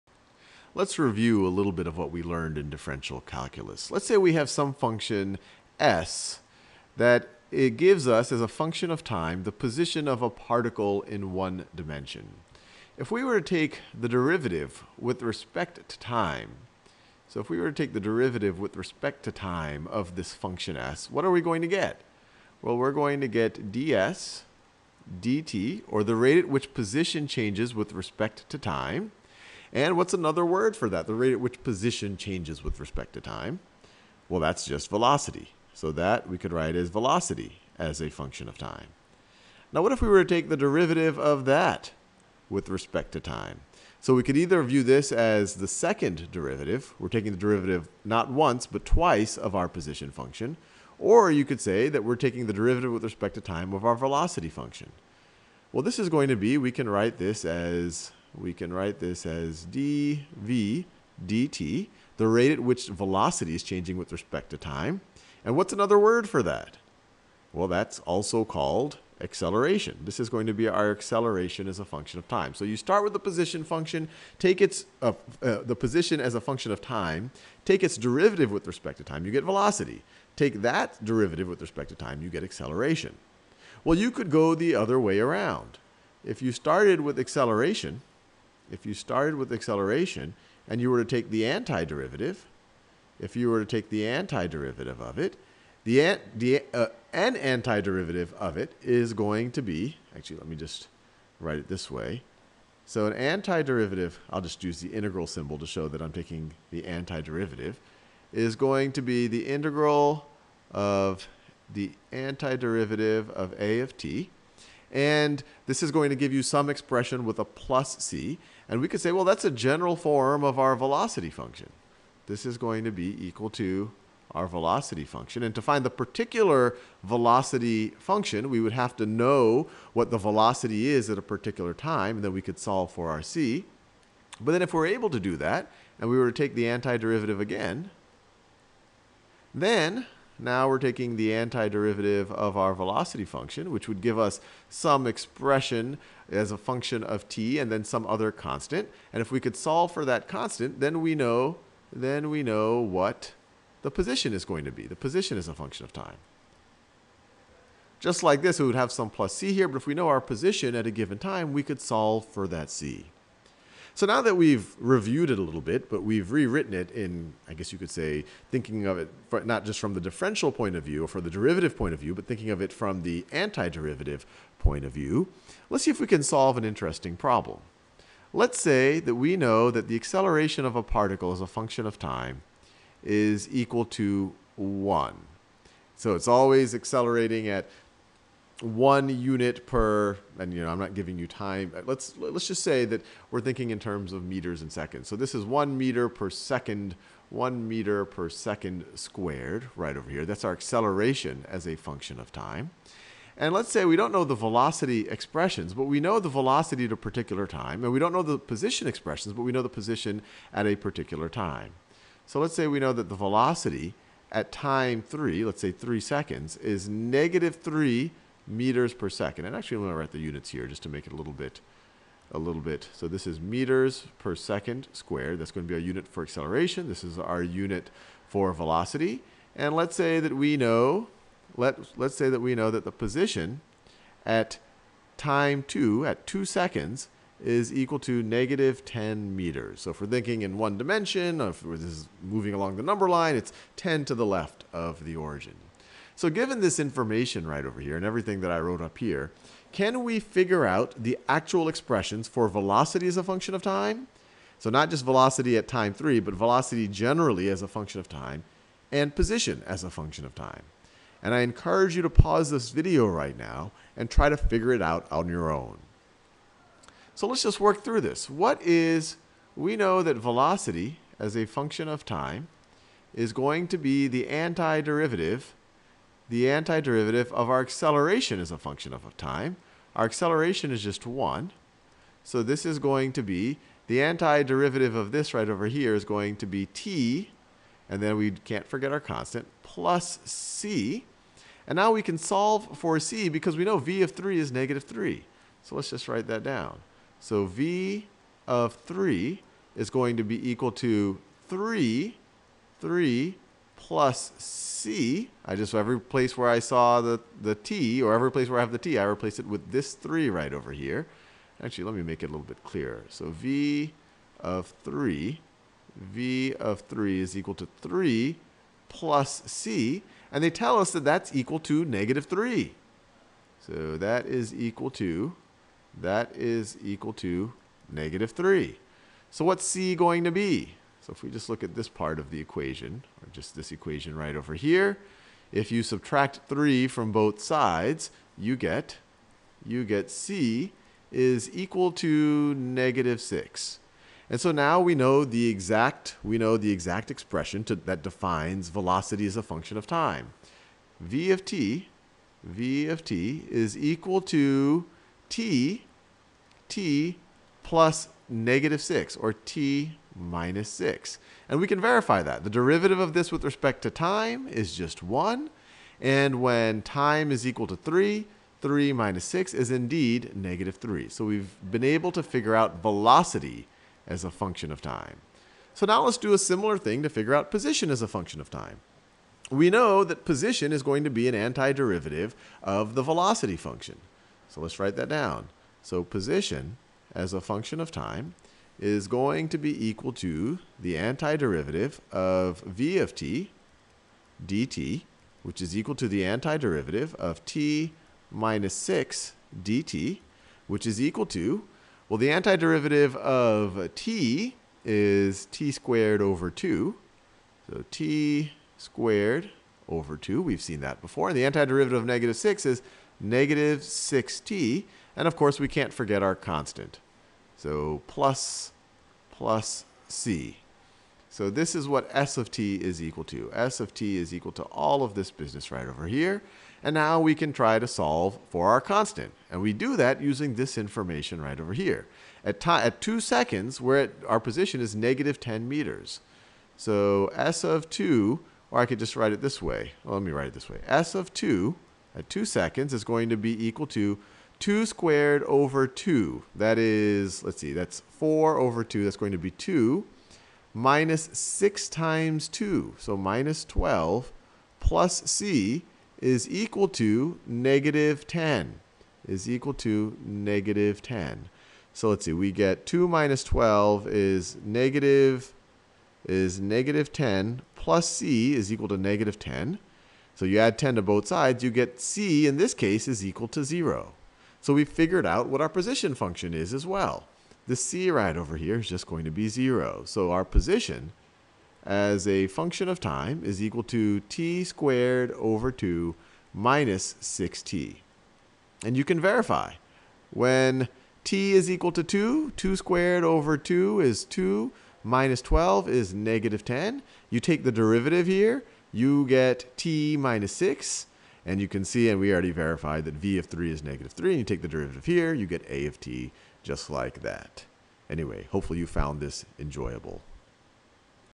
בעיה לדוגמא - בעיית תנועה עם אינטגרל מסוים
Worked example: motion problems (with definite integrals) | AP Calculus AB | Khan Academy Hebrew